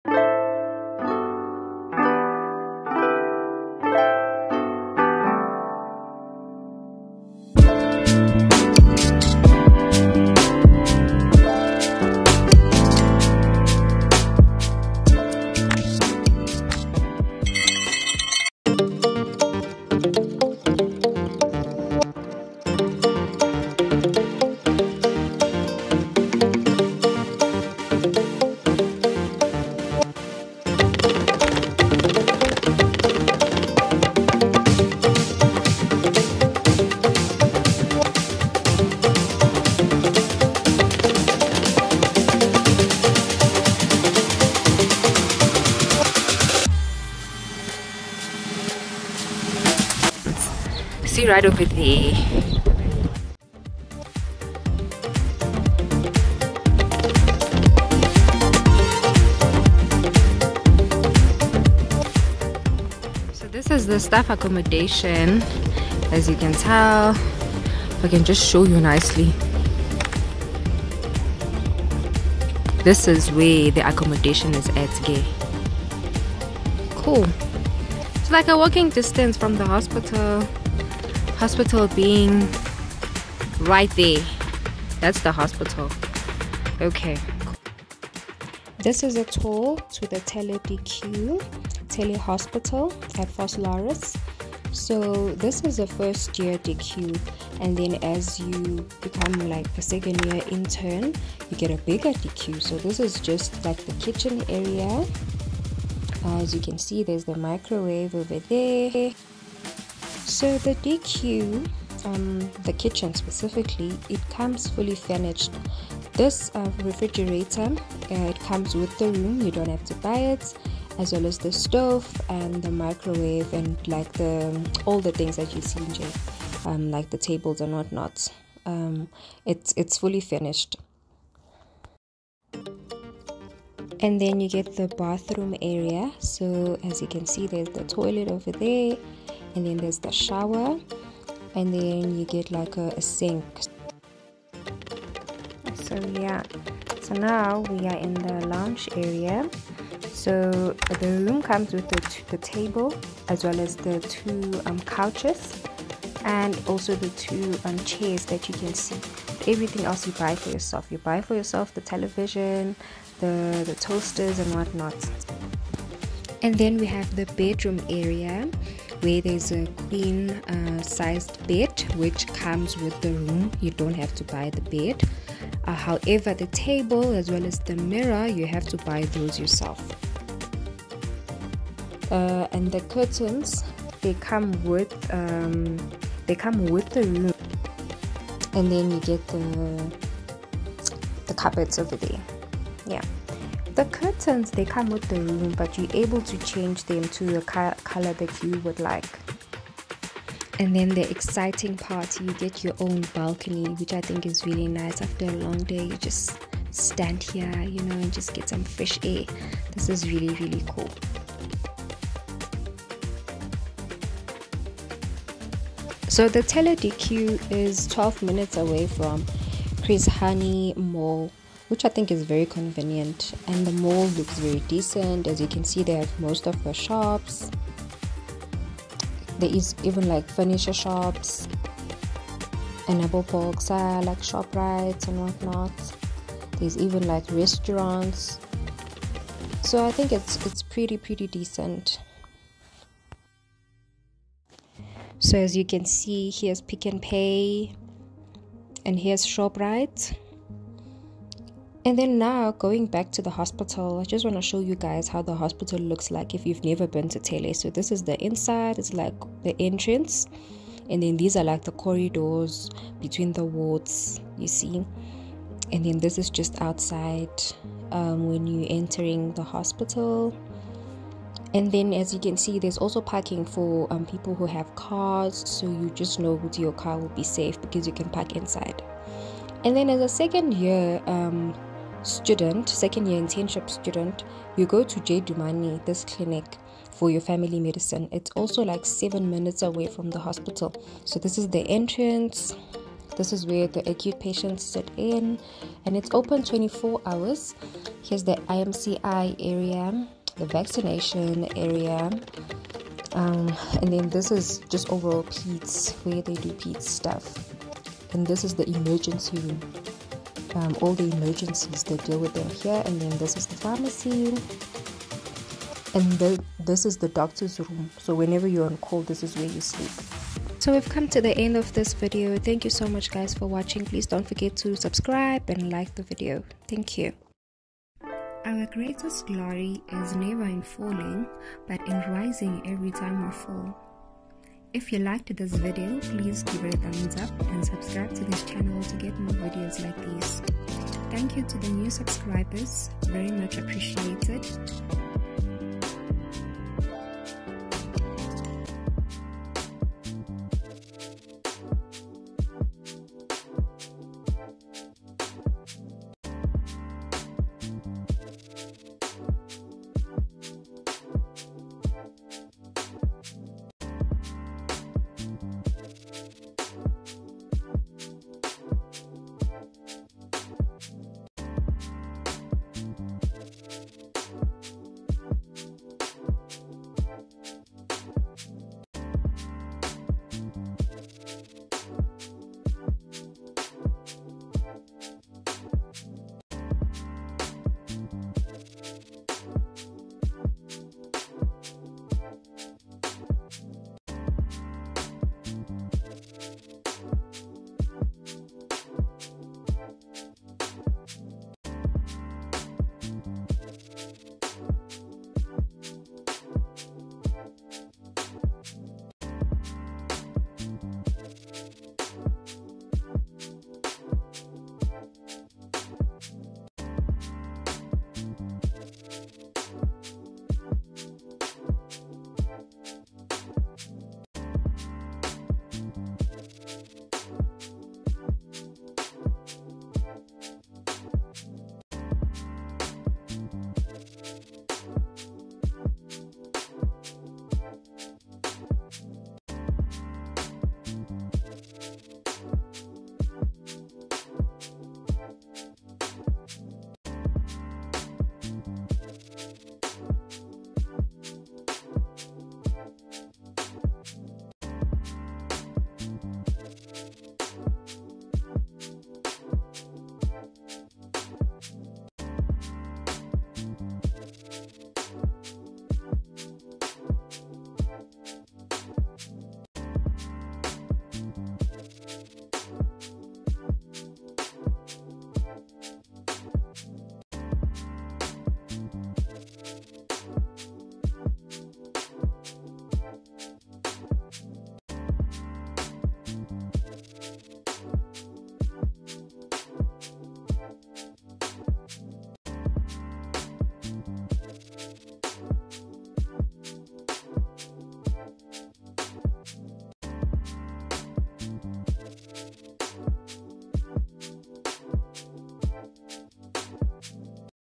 Thelle Mogoerane Regional Hospital DQ | Medical Doctor Internship Accommodation | R1000 RENT
Thelle Mogoerane Regional Hospital DQ | Medical Doctor Internship Accommodation

I am a medical student at the University of the Witwatersrand, I am with my group members at the hospital preparing to be doctors, watch me getting closer and closer to my dream of being a doctor, thank you so much for watching this video.

WAPT Study Planner